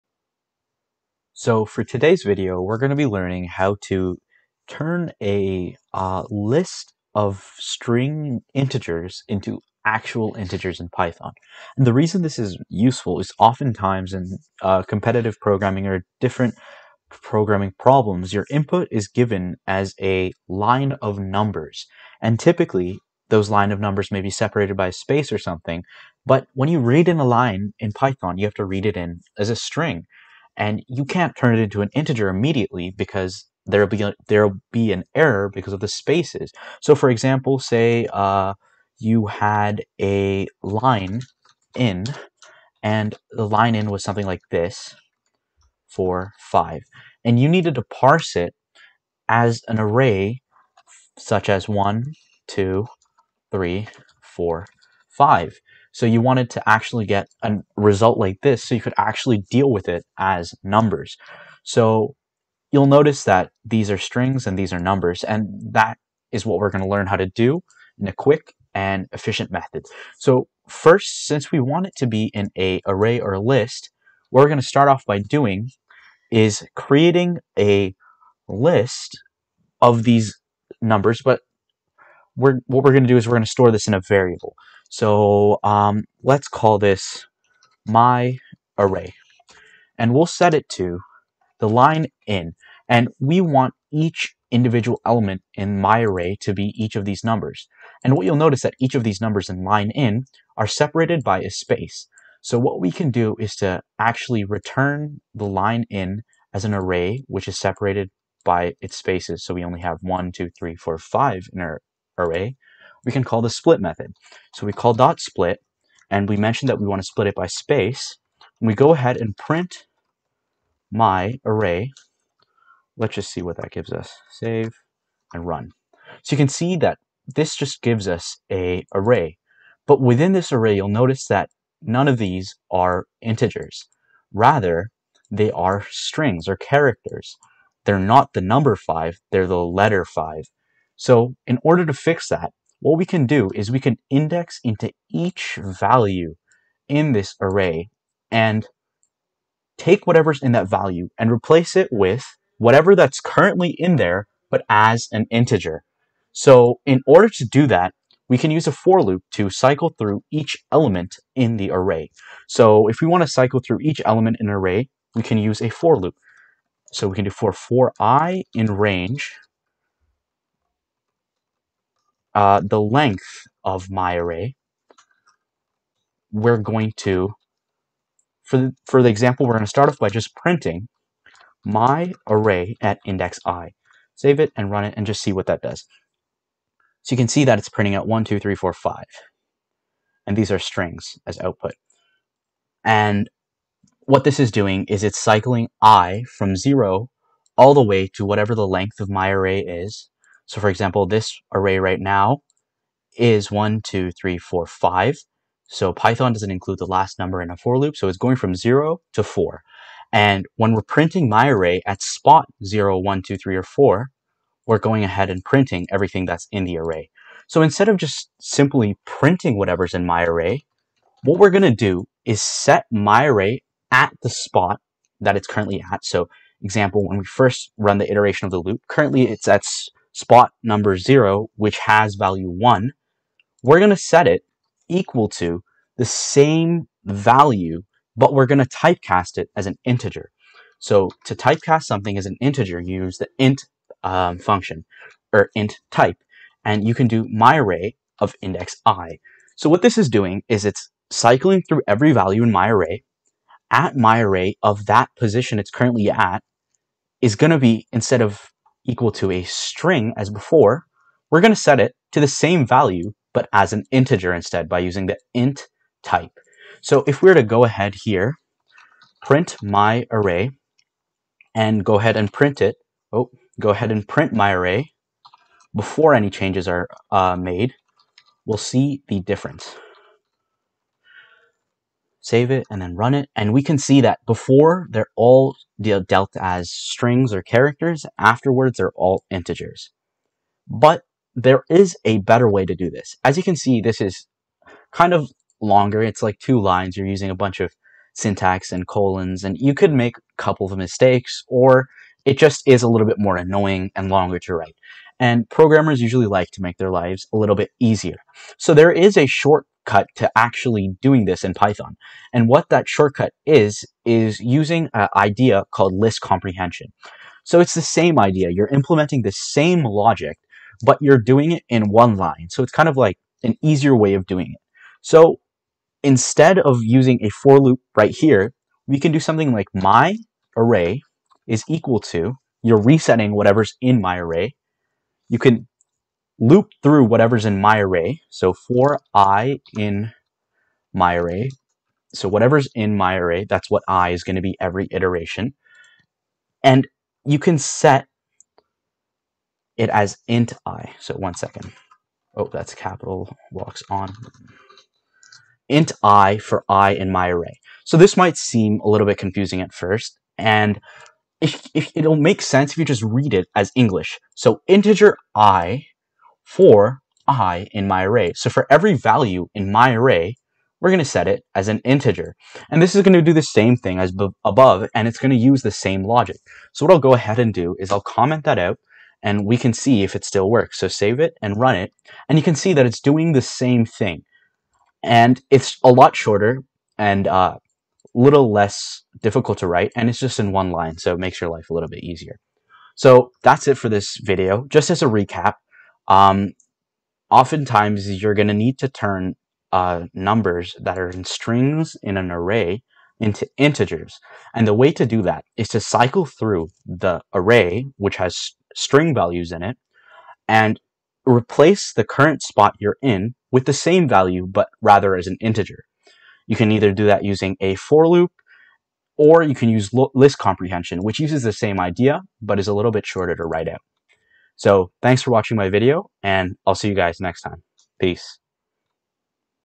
Python Tips: Converting a string list to an integer list, in 1 line
A simple easy way to turn a list of strings into integers in  python, all in one line.